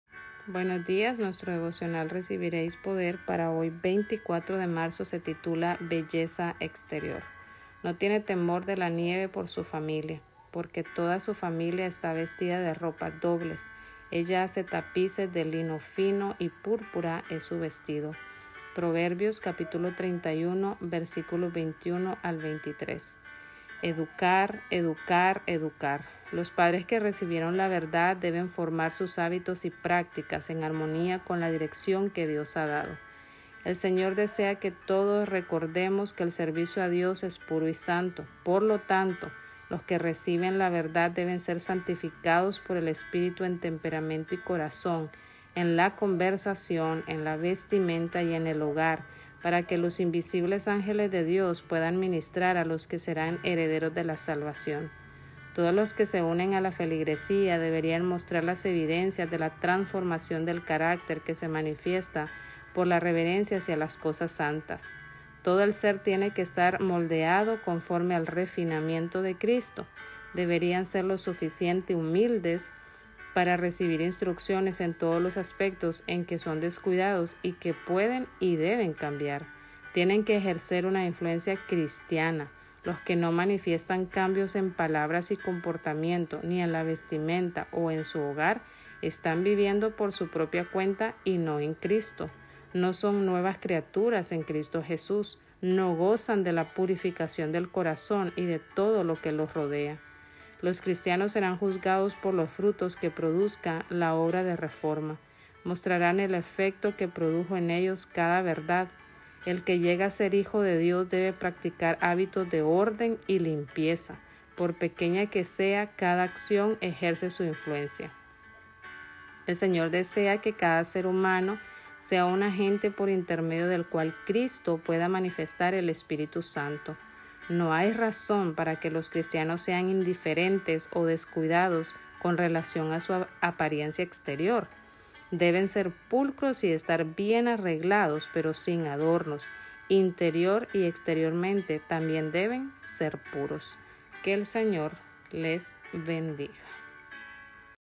RECIBIREIS PODER 24 de Marzo
Narrado por: Anayansi Aguilar
Belleza exterior, 24 de marzo

No tiene temor de la nieve por su familia, porque toda su familia está vestida de ropas dobles. Ella hace tapices; de lino fino y púrpura es su vestido. Proverbios 31:21-23.

Educar, educar, educar. Los padres que recibieron la verdad deben formar sus hábitos y prácticas en armonía con la dirección que Dios ha dado. El Señor desea que todos recordemos que el servicio a Dios es puro y santo. Por lo tanto, los que reciben la verdad deben ser santificados por el Espíritu en temperamento y corazón, en la conversación, en la vestimenta y en el hogar, para que los invisibles ángeles de Dios puedan ministrar a los que serán herederos de la salvación.

Todos los que se unen a la feligresía deberían mostrar las evidencias de la transformación del carácter, que se manifiesta por la reverencia hacia las cosas santas. Todo el ser tiene que estar moldeado conforme al refinamiento de Cristo. Deberían ser lo suficiente humildes para recibir instrucciones en todos los aspectos en que son descuidados, y que pueden y deben cambiar. Tienen que ejercer una influencia cristiana. Los que no manifiestan cambios en palabras y comportamiento, ni en la vestimenta o en su hogar, están viviendo por su propia cuenta y no en Cristo. No son nuevas criaturas en Cristo Jesús. No gozan de la purificación del corazón y de todo lo que los rodea.

Los cristianos serán juzgados por los frutos que produzca la obra de reforma. Mostrarán el efecto que produjo en ellos cada verdad. El que llega a ser hijo de Dios debe practicar hábitos de orden y limpieza. Por pequeña que sea, cada acción ejerce su influencia. El Señor desea que cada ser humano sea un agente por intermedio del cual Cristo pueda manifestar el Espíritu Santo. No hay razón para que los cristianos sean indiferentes o descuidados con relación a su apariencia exterior. Deben ser pulcros y estar bien arreglados, pero sin adornos. Interior y exteriormente también deben ser puros.—Testimonies to Southern Africa, 87. RP 94.4